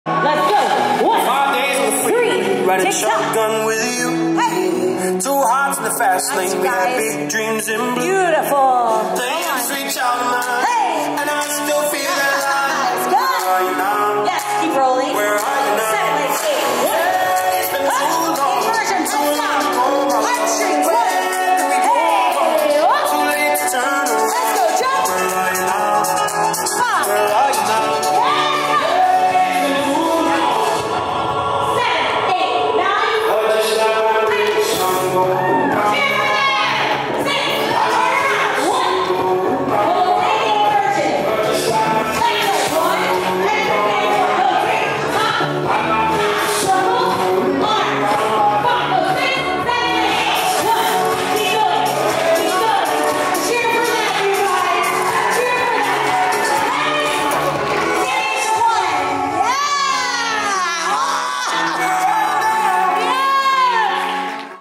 Celebrity Conventions: Jackie Nowicki
These teens showed off their athleticism with this amazing combo from @jackie_nowicki at Celebrity Conventions Dallas!🤩 Watch now!💗